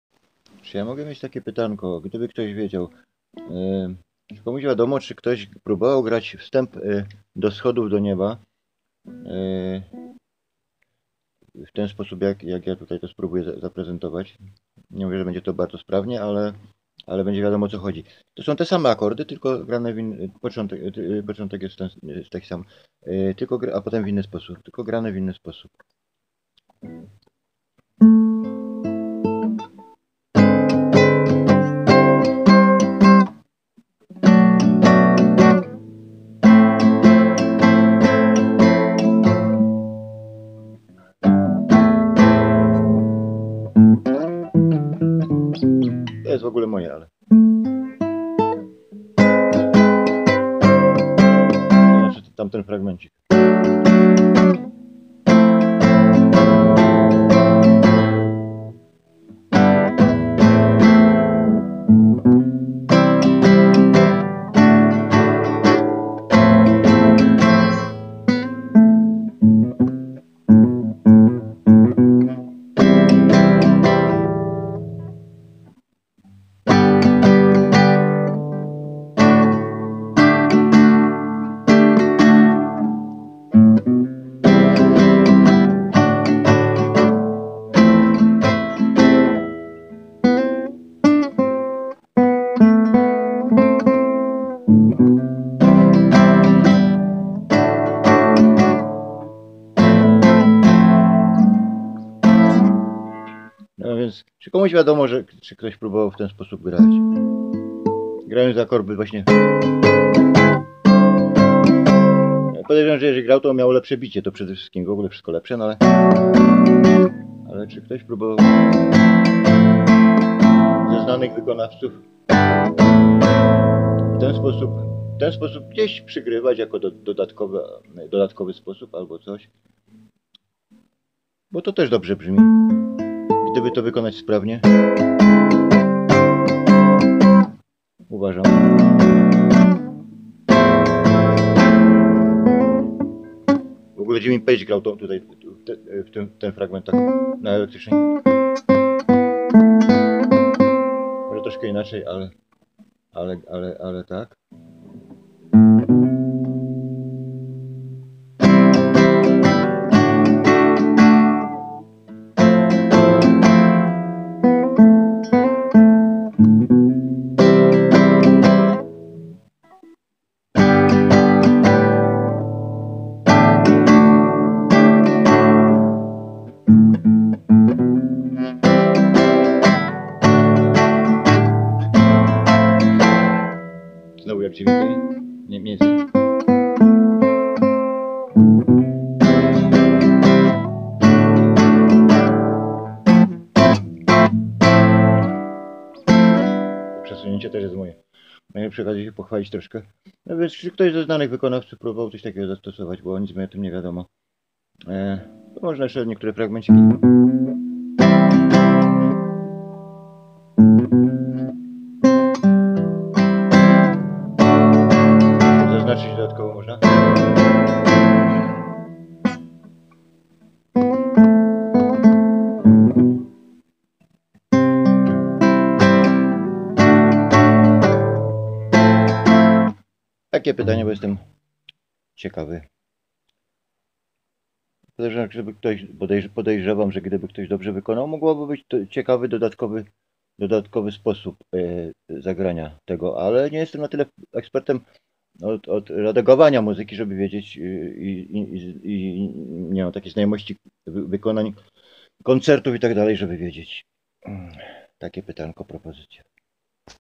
❓- Pytanie do ekspertów muzycznych.Moje przeprosiny,że nie widać całej gitary(jest ładna,hi,hi).📝👇
Będę wdzięczny, jeżeli skomentuje ktoś, kto wie w tym względzie więcej, niż ja, być może osoba grająca na gitarze.Czy ktoś oprócz mnie uznał taki sposób gry za ciekawy.Chcę zaznaczyć, że moje słowa na video " to akurat jest moje" , dotyczą tylko paru dźwięków basowych ( tych ze slajdem).To mój sposób gry tego fragmentu.